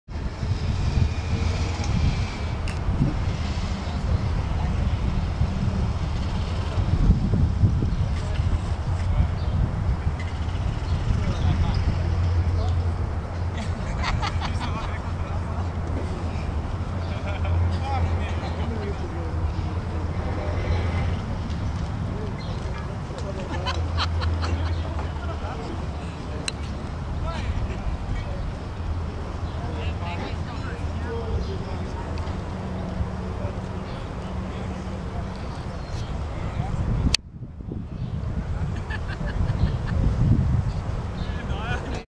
Coetzee-Basson-Kruger playing around.mov
Christiaan Basson attempts to chip and catch - with Jbe Kruger and Charl Coetzee